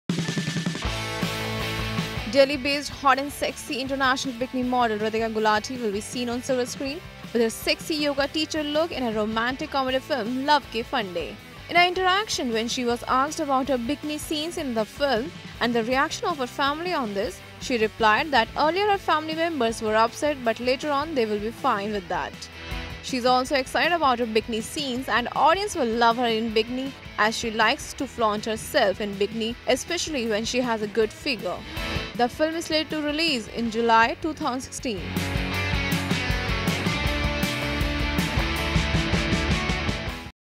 Sexy bombshell Ritika likes to flaunt herself in Bikni
Delhi based hot and sexy International Bikni model Ritkia Gulati will be seen on silver screen with her sexy yoga teacher look in a romantic comedy film 'Love ke Funday'.